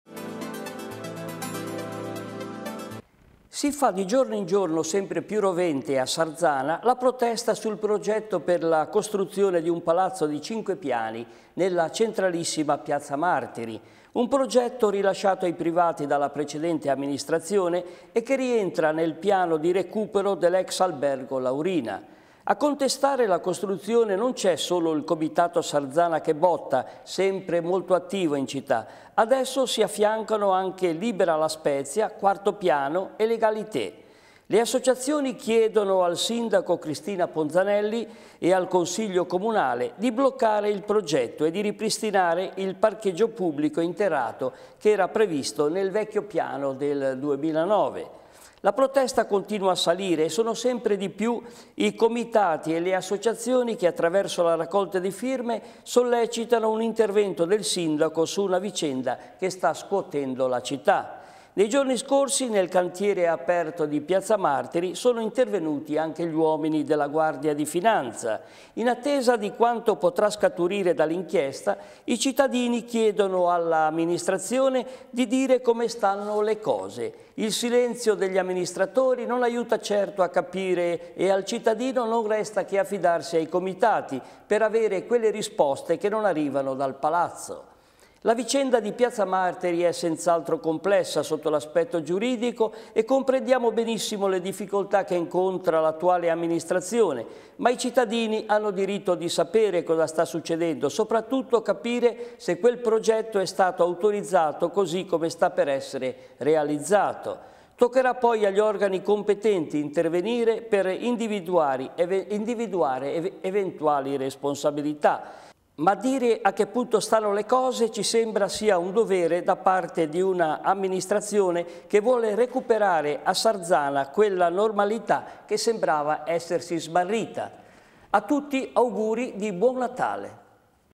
Nota della settimana, il palazzo dietro il Laurina a Sarzana 21-12-2018
Come ogni venerdi la rubrica di Riccardo Sottanis "la nota della settimana".
Questa sera si occupa delle polemiche collegate alla piazza Martiri a Sarzana.